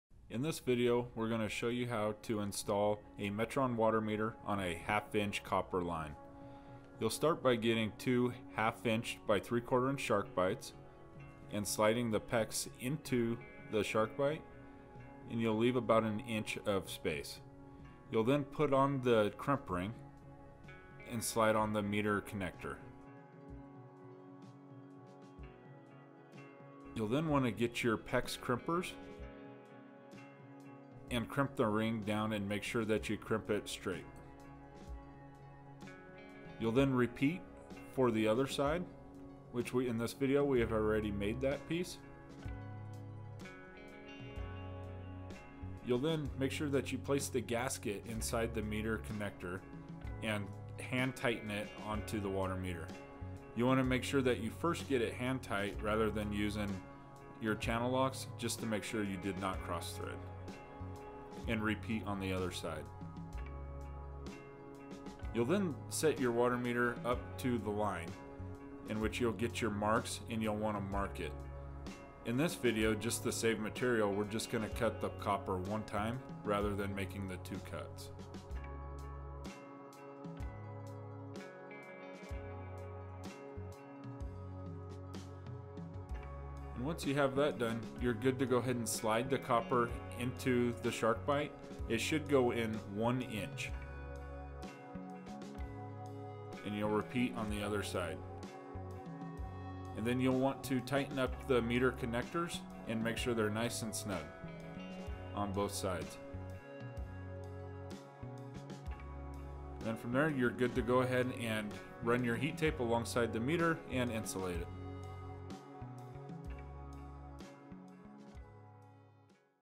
Water Meter Installation - 1/2" Copper Pipe
In this video we discuss the process in installing a Metron Spectrum 30 residential water meter in a 1/2" copper pipe. This video is also applicable to installing other types of water meters in a copper pipe.